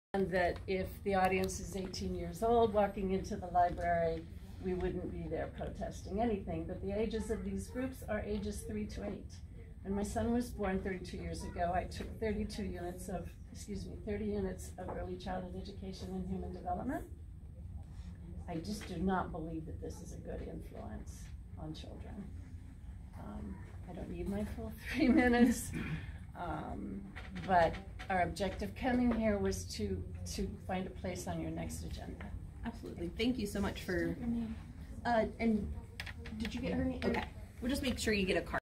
LA County MassResistance Leader Speaks Out Against
10.14.19.

MassResistance chapter chair Cheryl Wendell speaks out against Drag Queen Story Hour coming to the city of Torrance.